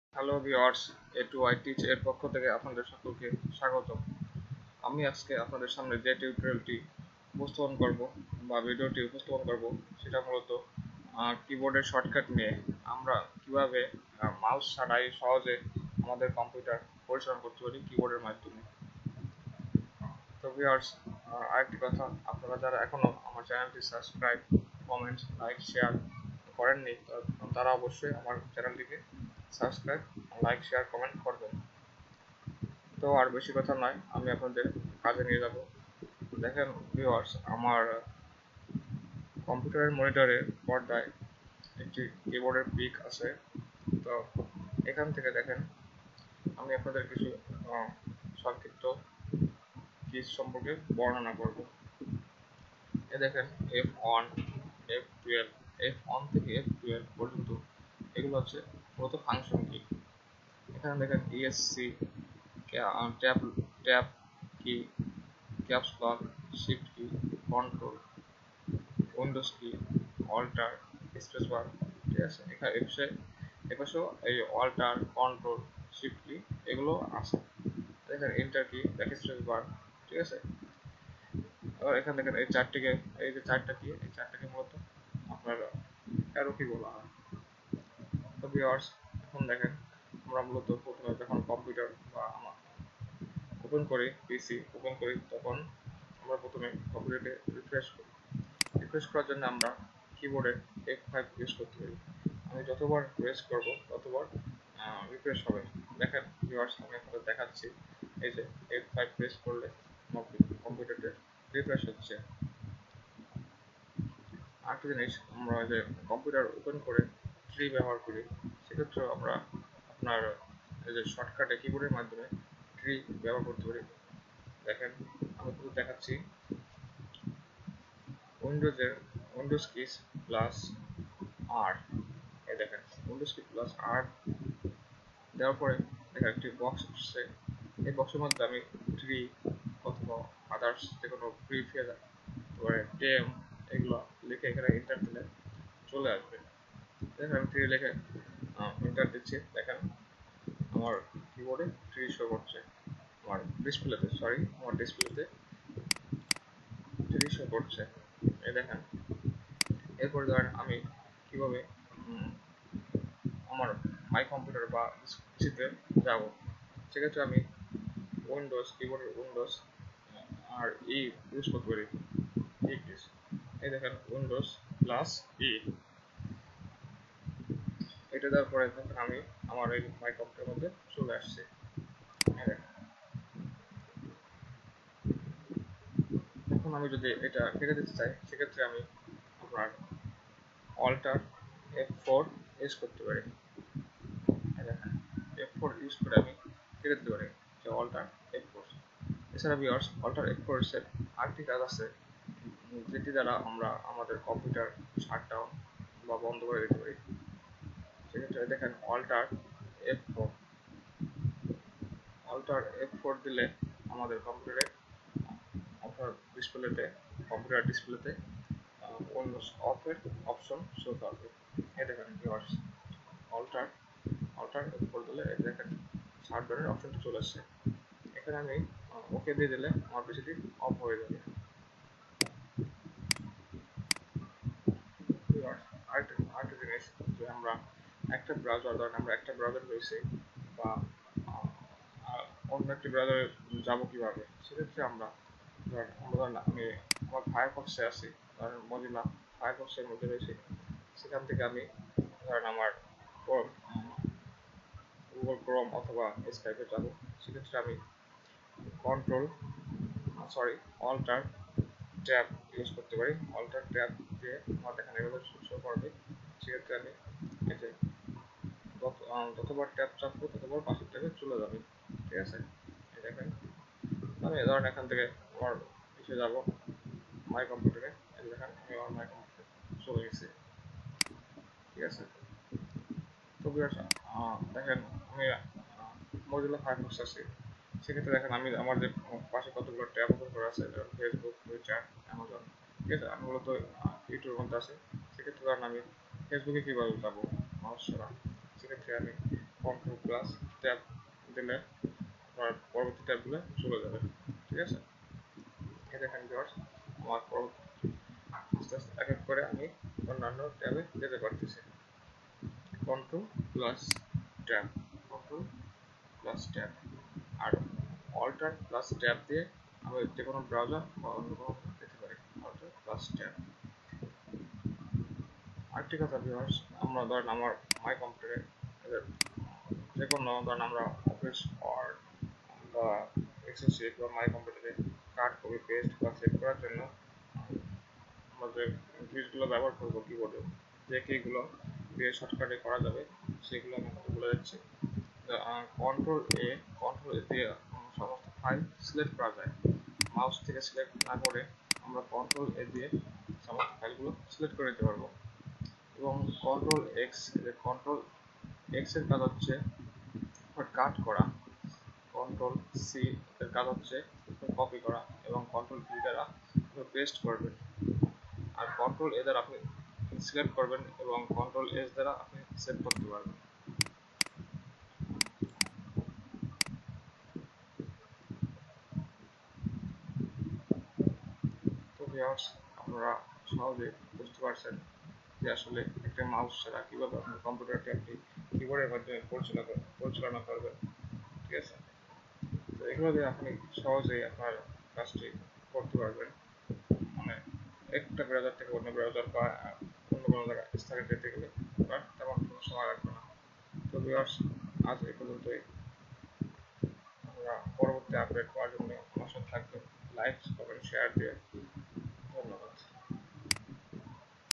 how to use computer without mouse !! key-board shortcuts !!
Most used and important shortcut keys for any windows. Computer shortcuts key that will help you more to effective and expert computer using. Windows shortcut keys for expert computer, Laptop, PC user………